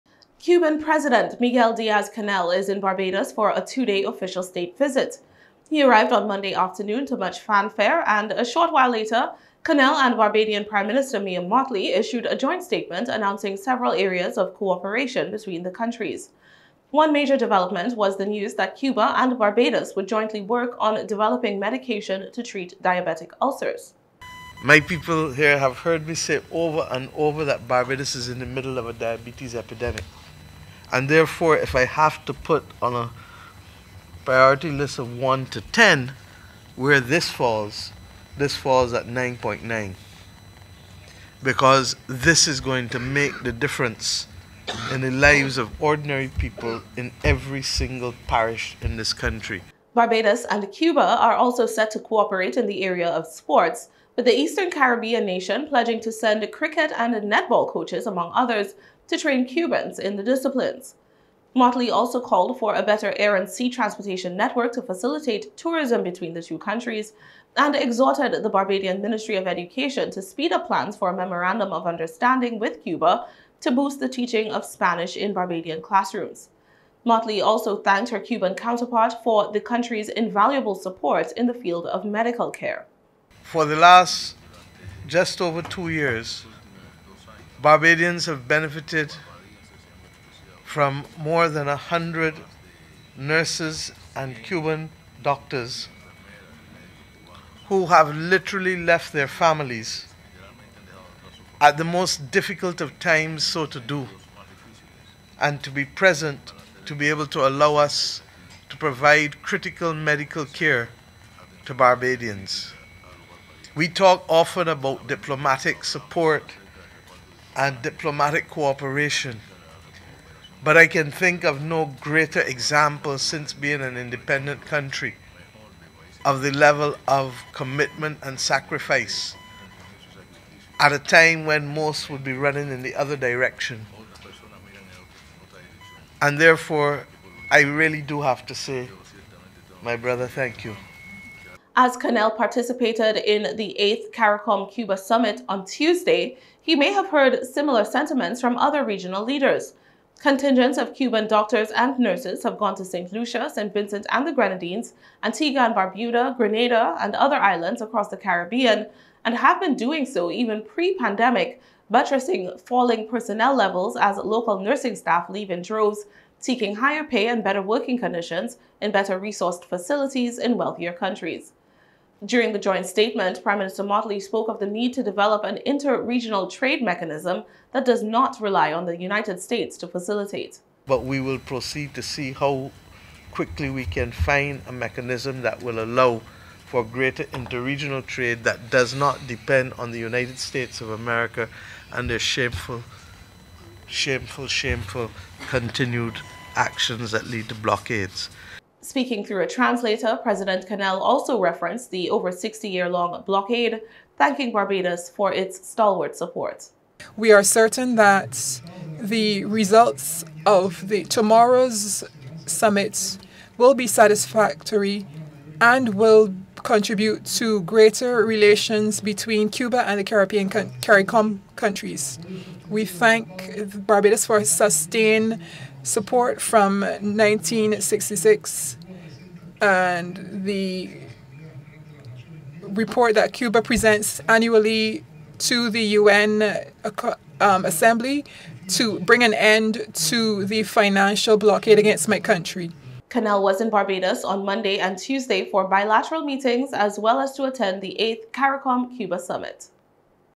Cuban President in Barbados for 2-Day State Visit, Will Attend 8th CARICOM-Cuba Summit
Cuban President Miguel Diaz Canel is in Barbados for a 2-day official state visit. He arrived on Monday afternoon to much fanfare, and a short while later, Canel and Barbadian Prime Minister Mia Mottley issued a joint statement announcing several areas of co-operation between the countries.